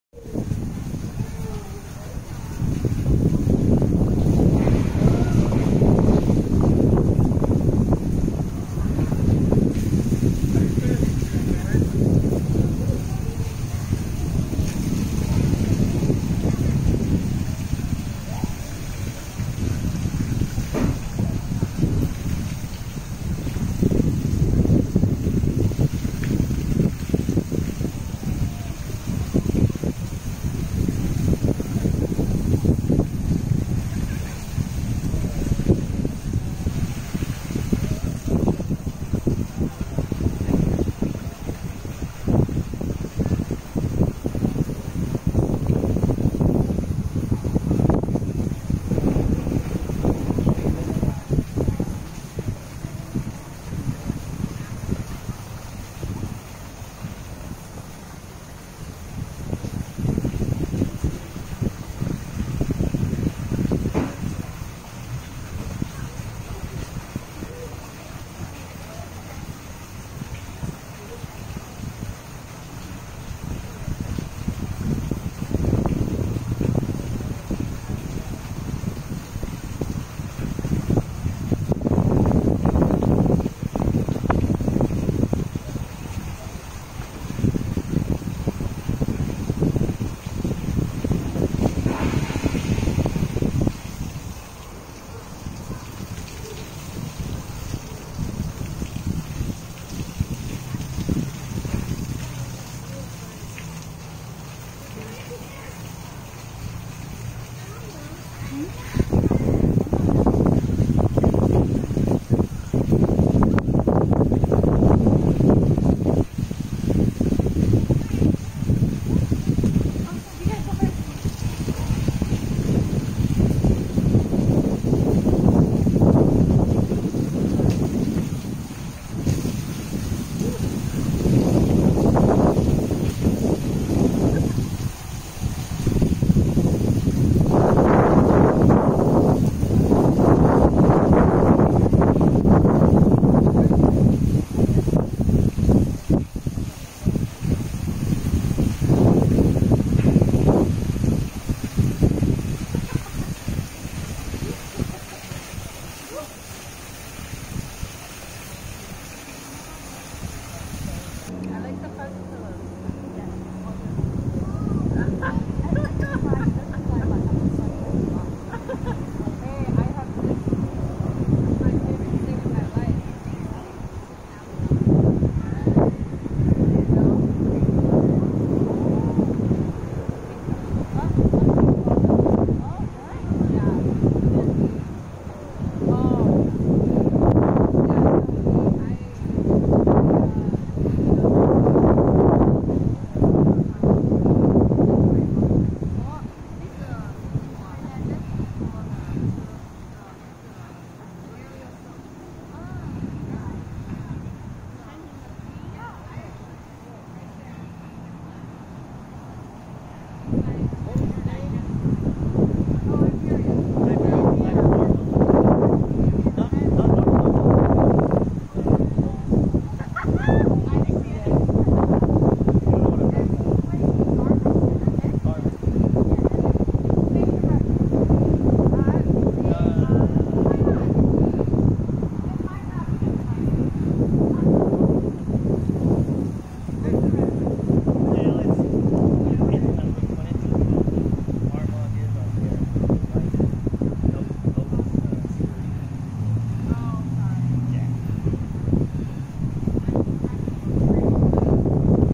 Rest Room Stop other end of Sharks Cove.
Pupukea, aka Sharks Cove  beautiful snorkeling area.